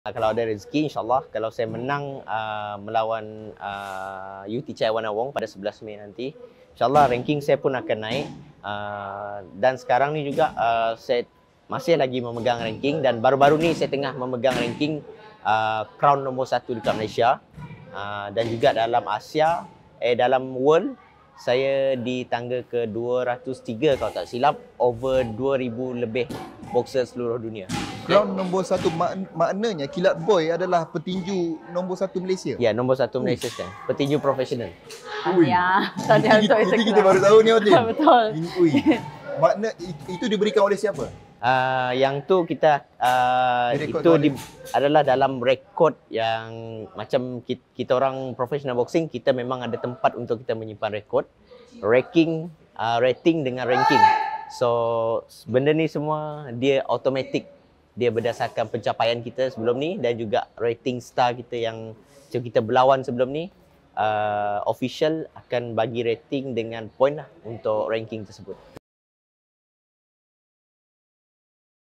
Kilat Boy pegang ranking petinju no.1 Malaysia | TVS
Petinju profesional Sarawak, Daeloniel Mcdelon Bong atau lebih dikenali dengan ‘Kilat Boy’ kini memegang ranking petinju nombor satu di Malaysia.